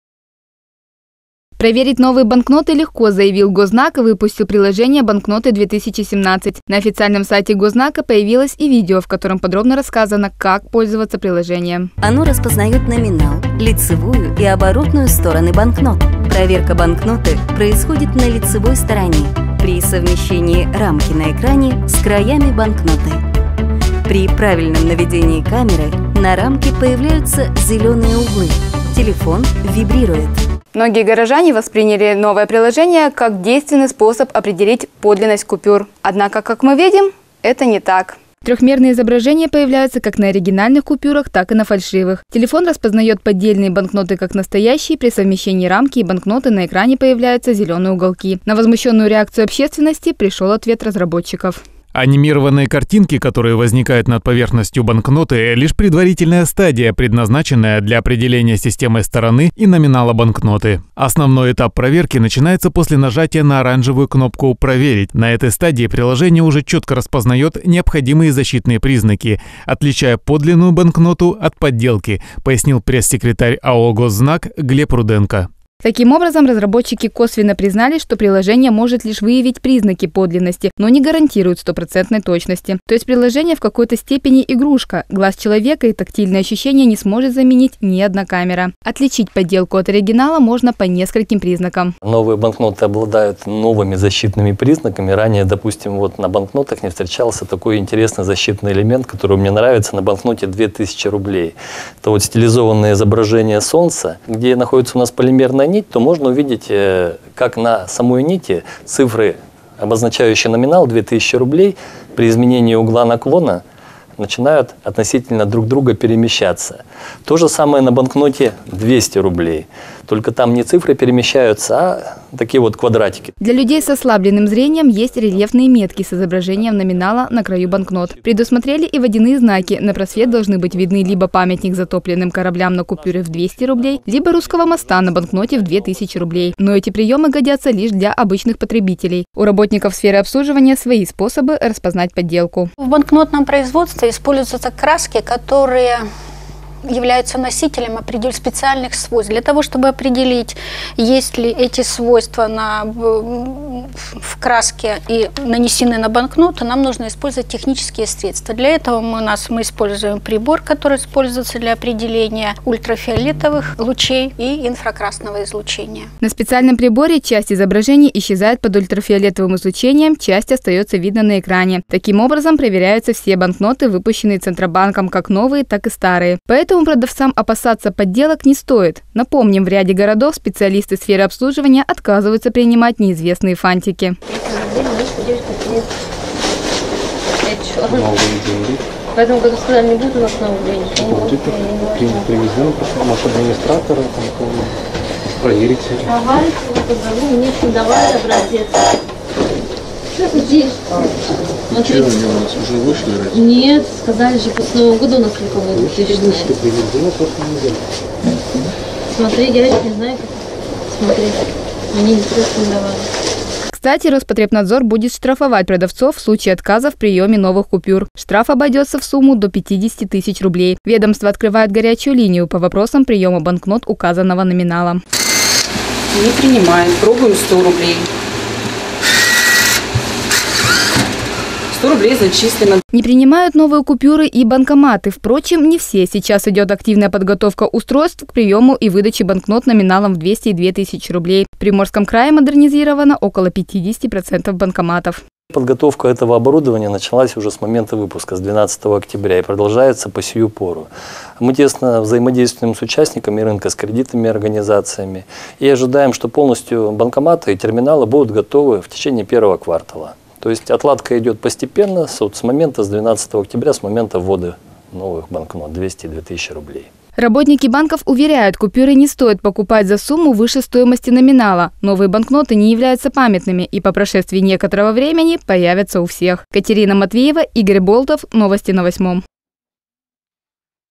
Банкноты-2017: игрушка или инструмент?
Гознак выпустил мобильное приложение, позволяющее определять подлинность недавно выпущенных новых банкнот - в 200 и 2000 рублей. Одной из причин разработки приложения стал регулярный отказ продавцов принимать подобные деньги.